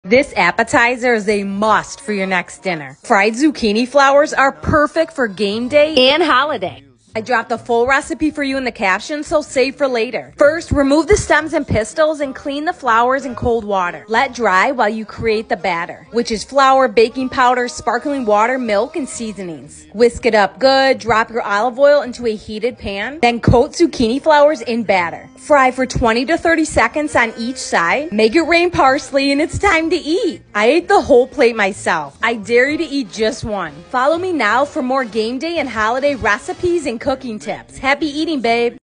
fried zucchini flowers 🇮🇹🌼are the perfect appetizer you must make 🥰 try it w/my easy recipe↘️
fried zucchini flowers 🇮🇹🌼are the perfect holiday + game day appetizer you must make 🥰 try it 2day w/my easy recipe below…

8 zucchini flowers (stems + pistels removed)
1/4 tsp baking powder
1/3 c AP flour
1 tsp 2% milk
1/3 c sparkling water
2 pinches sea salt
1 pinch black pepper
1 pinch garlic power
1 pinch onion power
1 pinch cayenne pepper

Start by filling a medium bowl w/cold water. Next, gently & carefully remove stems & pistels from flowers. Then gently split flower petal in half w/your hands. Repeat until all petals are halved.

Submerge your halved petals into cold water making sure water covers them completely. Then grab each petal & use your fingers to gently remove orange pollen & dirt.

Place on a paper towel lined baking sheet & let dry for 20mins. Then gently turn petals over to the other side & let dry for another 20mins.

While you’re waiting for the petals to dry, add to a medium bowl 1/3 c AP flour, 1/4 tsp baking powder, 2 pinches sea salt, 1 pinch black pepper, 1 pinch garlic power, 1 pinch onion power, 1 pinch cayenne pepper, 1 tsp 2% milk & a little sparkling water at a time (1/3 c sparkling water total).

Whisk together until mixture thickens to a batter. Let batter sit while petals dry.

Next, line a baking sheet w/paper towel. This is where you will place your cooked zucchini flowers after cooking in order to remove excess oil.

Once the petals are dried, place a deep frying pan on medium high heat. Let heat up for 1min then cover the pan bottom w/olive oil. Let oil heat up for 2-3mins.

Dip the zucchini flowers into the batter, ensuring it’s completely coated w/batter.

Gently place battered zucchini flowers into the hot oil AWAY from you (to avoid oil splashing towards you).

Cook for about 20-30secs on one side & then flip & cook for another 20-30secs.

Place cooked zucchini flowers onto a tray lined w/paper towel to remove excess oil.

Garnish w/fresh chopped Italian parsley & it’s onnn 🍽️ make these delish appetizers today for your next dinner or party every1 will love them 😬

~save 4 later
~follow 4 more holiday+game day recipes